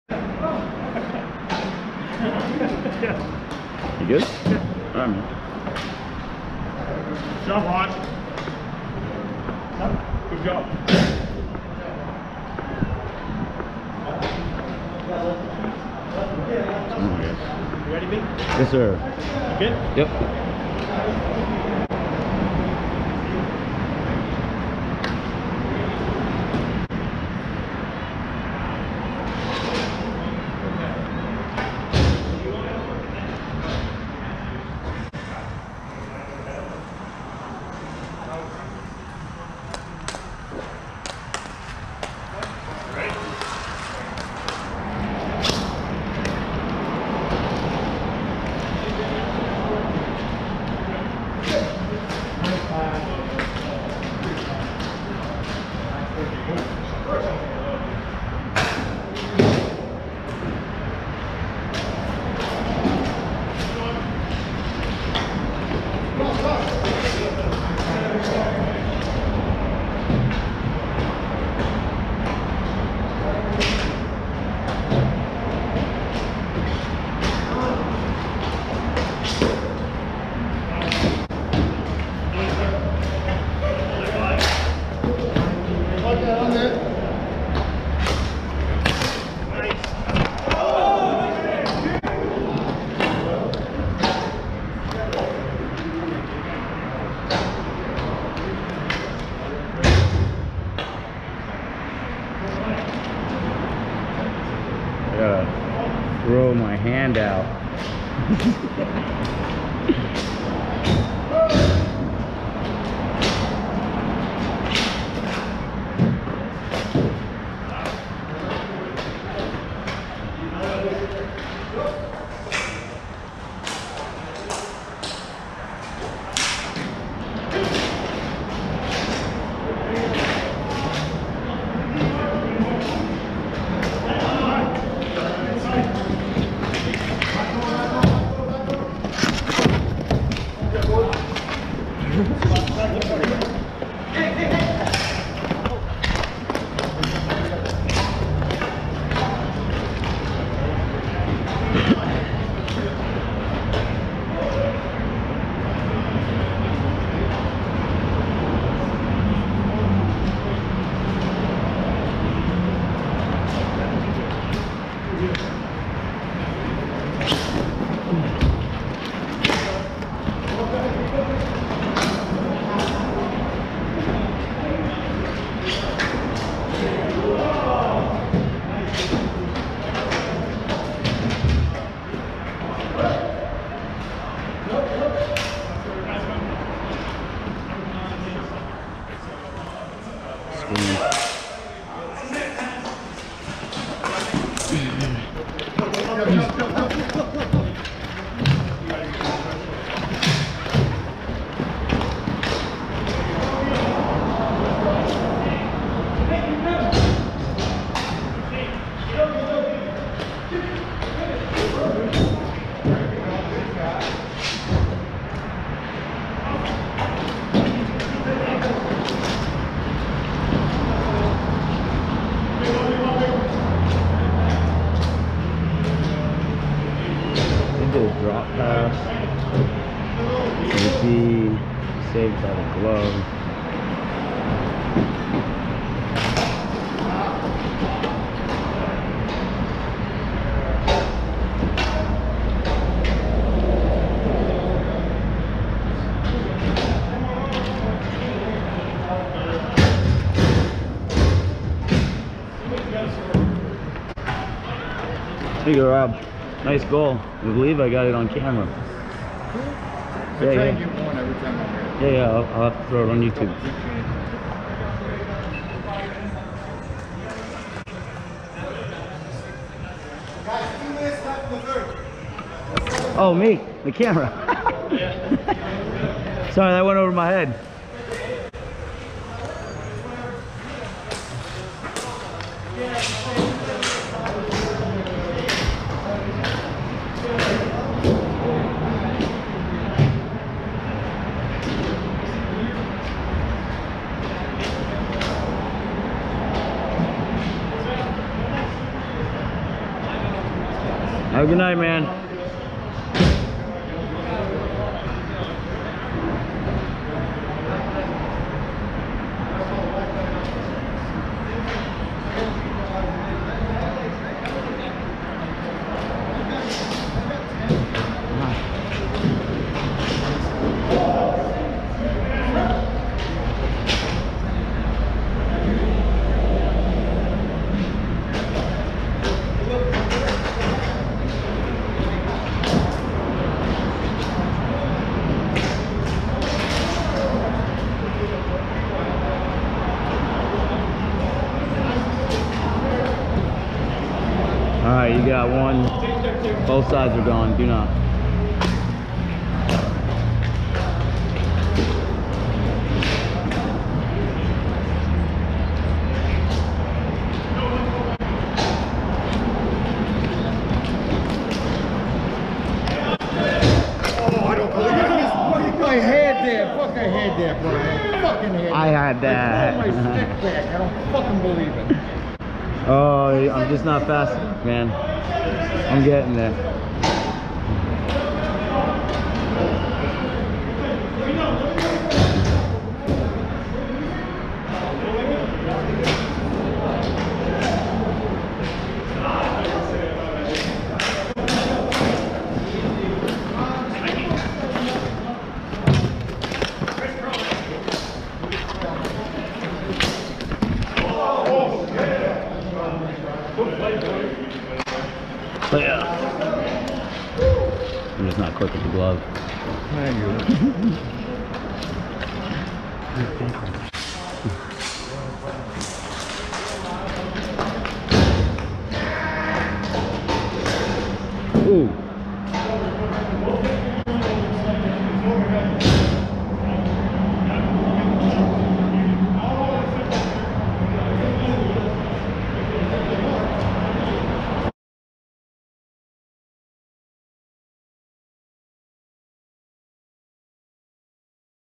11NOV2021 Game 2 I couldn't make a single save!
The previous video uploaded had a corruption about six minutes in.
I apologize, and hope to the four people who had seen it come back to watch again!

As the title implied, I make 0 saves